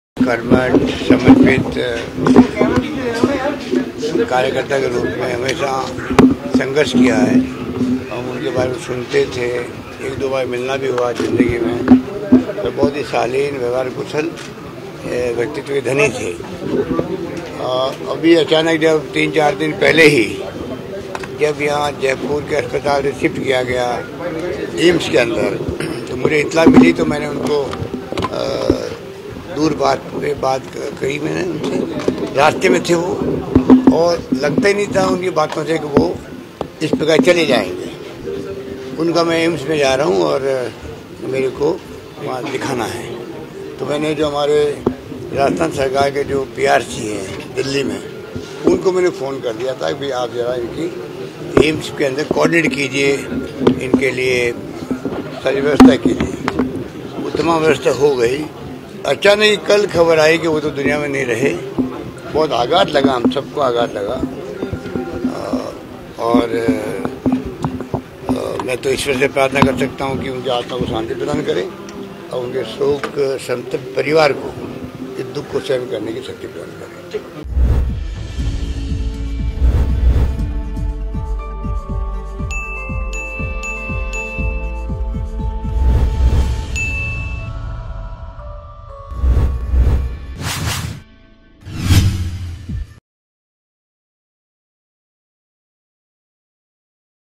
AJMER NEWS | भाजपा प्रदेशाध्यक्ष मदन लाल सैनी का निधन | MTTV INDIA
भाजपा प्रदेशाध्यक्ष मदन लाल सैनी का निधन
मुख्यमंत्री अशोक गहलोत ने कहा- दिल्ली जाने से पहले भी मैंने की थी सैनी से बात
दिल्ली के अधिकारियों को दिए थे वहां की व्यवस्था करने के निर्देश
सैनी ने जताया था जल्द स्वस्थ होकर वापस लौटने का भरोसा
मदनलाल सैनी जमीन से जुड़े नेता थे :मुख्यमंत्री
सैनी के निधन की सूचना से आघात लगा :मुख्यमंत्री
ईश्वर उनकी आत्मा को शांति प्रदान करें :मुख्यमंत्री